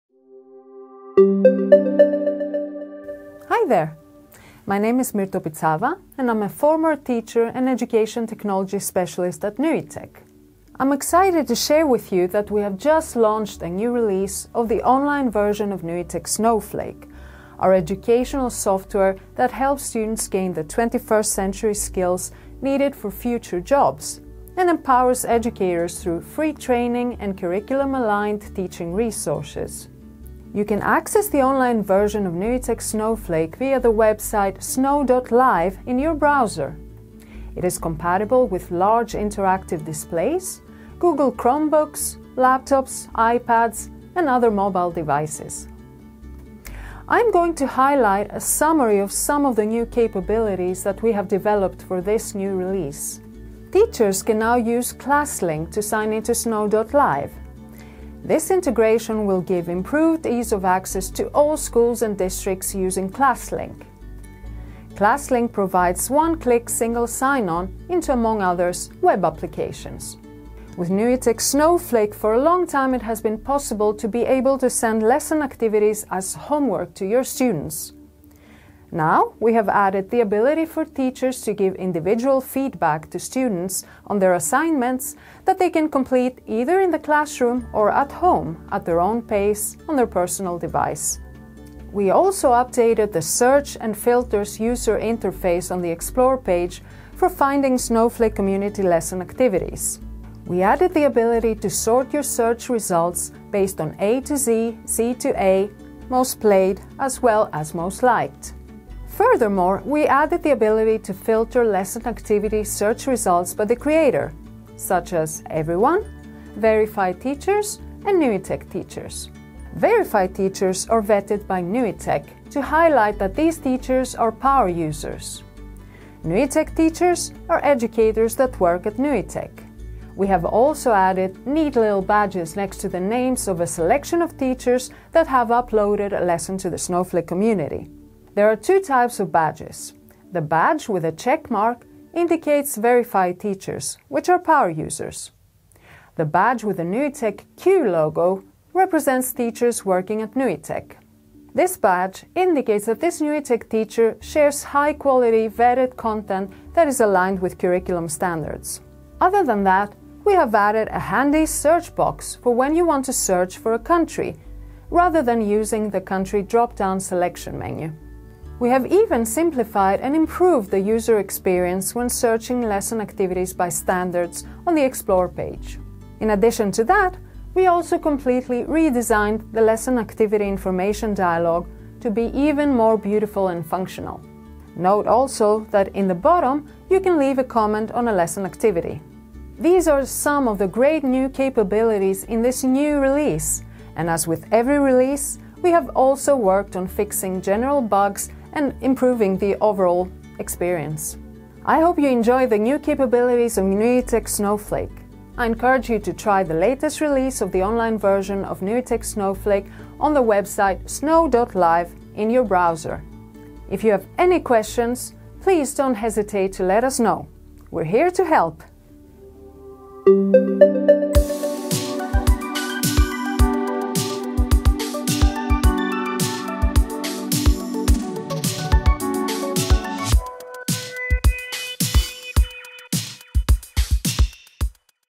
NUITEQ Snowflake - Web release
The latest release of NUITEQ Snowflake (web version), our educational software that brings interactivity to your school and boosts classroom engagement, is out today!

What's new?

- Individual feedback to students
- Sort and filter your search results
- Badges next to the names of a selection of teachers
- Search by standards or country
- Redesigned the lesson activity dialog
- Leave a comment on a lesson activity